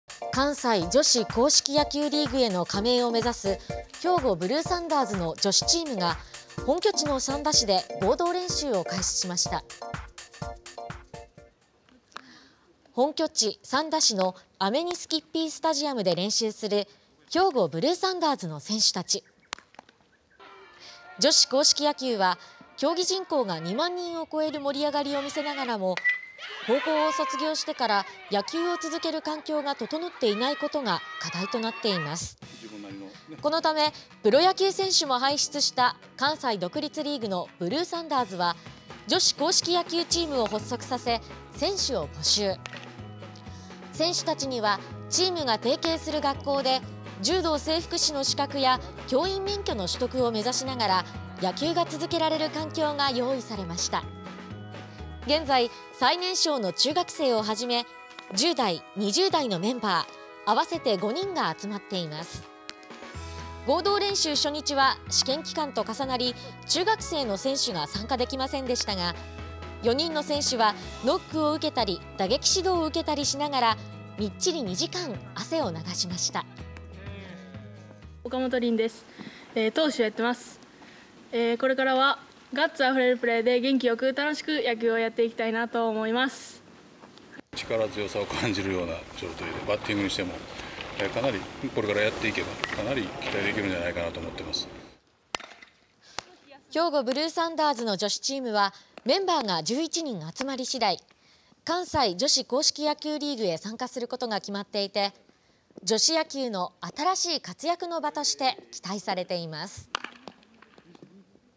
関西女子硬式野球リーグを目指せ 兵庫ブルーサンダーズ女子チーム発足
関西女子硬式野球リーグへの加盟を目指す兵庫ブルーサンダーズの女子チームが、本拠地の三田市で合同練習を開始しました。

本拠地・三田市のアメニスキッピースタジアムで練習する兵庫ブルーサンダーズの選手たち。

女子硬式野球は、競技人口が2万人を超える盛り上がりを見せながらも、高校を卒業してから野球を続ける環境が整っていないことが課題となっています。

このため、プロ野球選手も輩出した関西独立リーグのブルーサンダーズは、女子硬式野球チームを発足させ、選手を募集。 選手たちには、チームが提携する学校で柔道整復師の資格や教員免許の取得を目指しながら野球が続けられる環境が用意されました。

現在、最年少の中学生をはじめ10代、20代のメンバー合わせて5人が集まっています。

合同練習初日は、試験期間と重なり、中学生の選手が参加できませんでしたが、4人の選手はノックを受けたり、打撃指導を受けたりしながらみっちり2時間汗を流しました。

兵庫ブルサンダーズの女子チームはメンバーが11人集まり次第、関西女子硬式野球リーグへ参加することが決まっていて、女子野球の新しい活躍の場として期待されています。